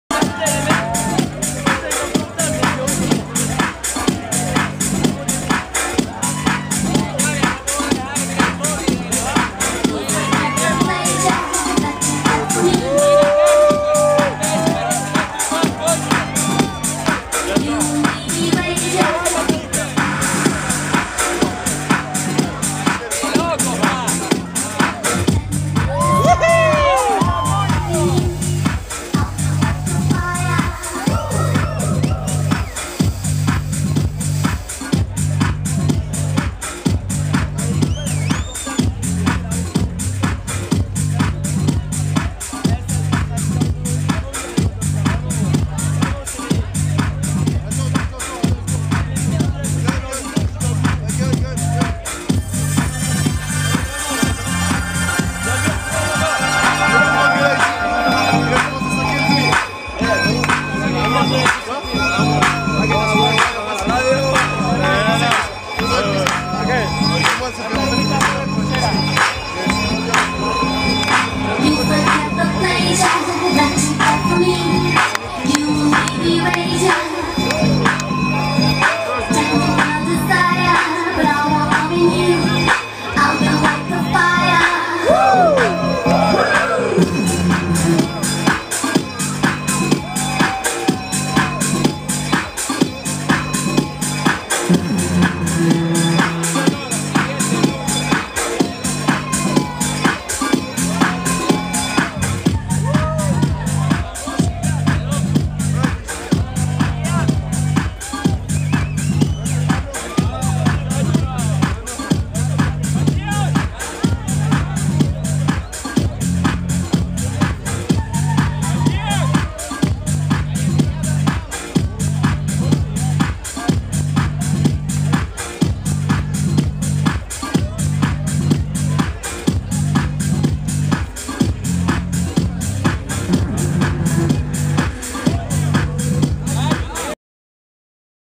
Matthias Tanzmann @ Mobilee Pool Session Sonar Off Barcelona 2014
video cedido por Lectrolukas